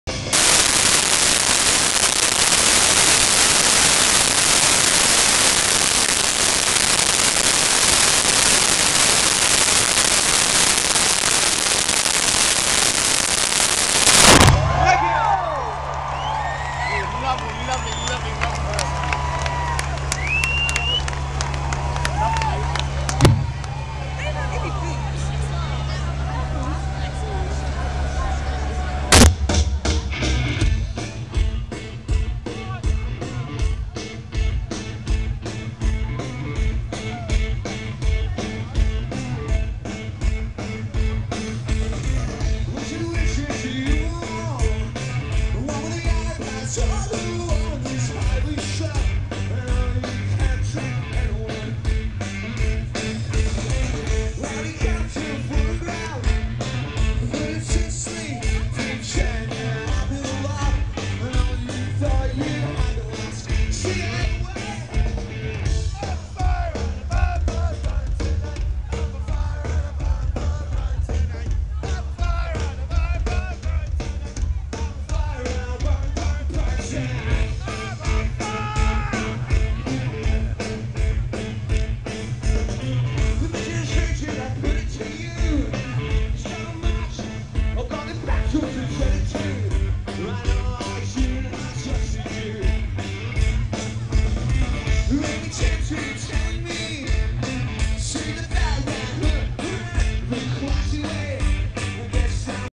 Isle of Wight 2010 - Muddy's on FIIIIIIIRE!!!!!
14secs of static (apologies), Then a bit of Biffy with the Muddster hisself doing IMHO a much better job!